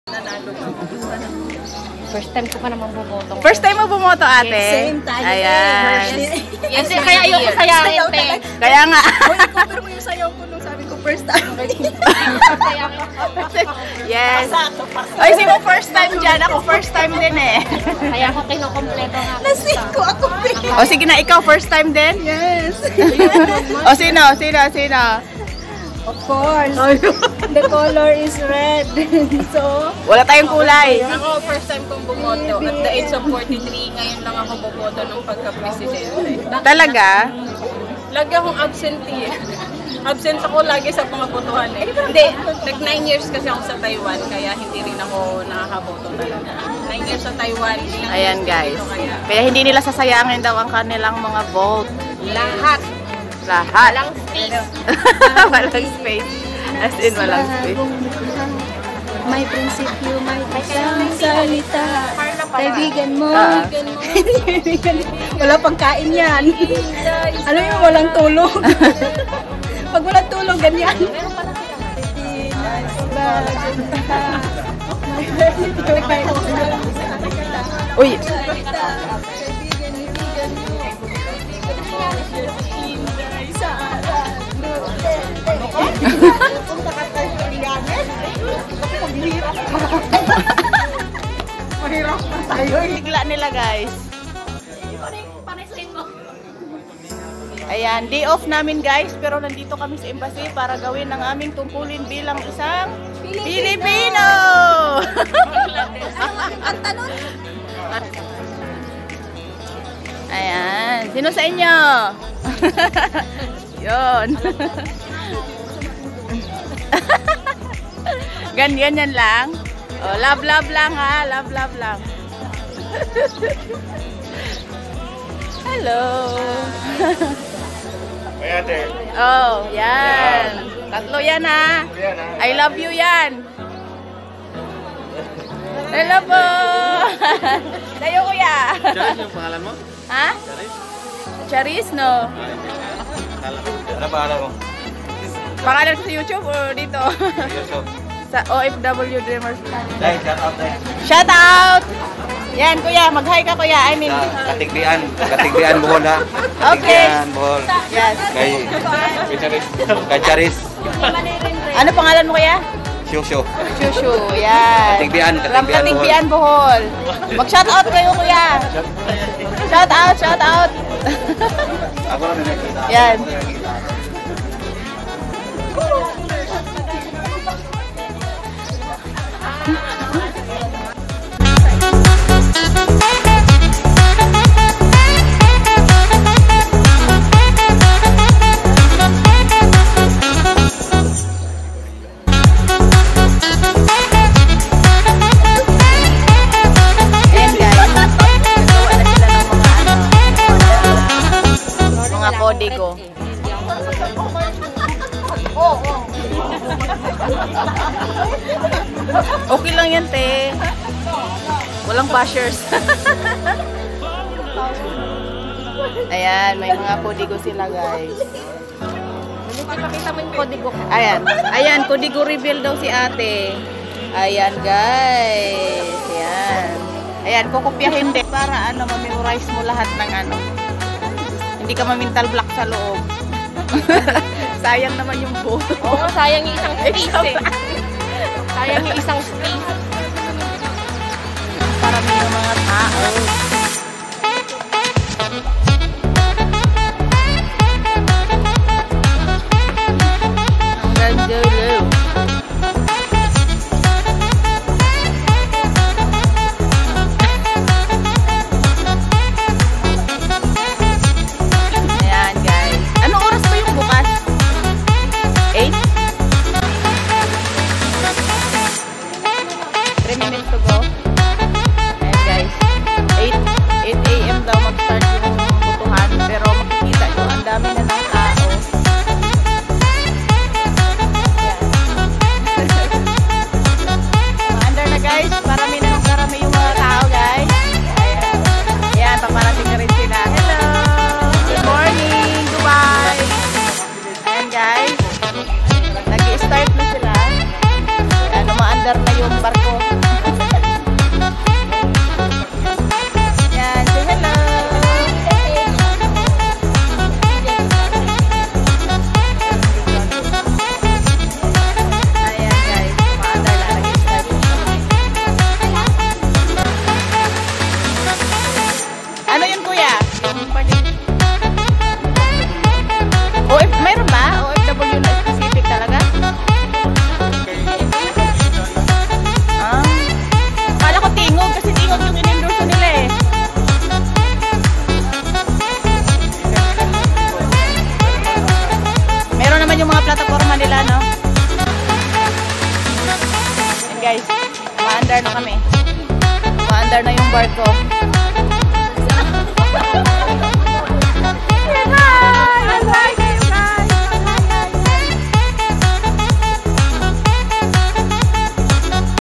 BAGONG BOTANTE NA OFW SA DUBAI/ Excited na bumuto. April 24, 2022.
naging masigla at excited ang mga first time na boboto na mga kababayan natin sa dubai..  noong april 24, 2022.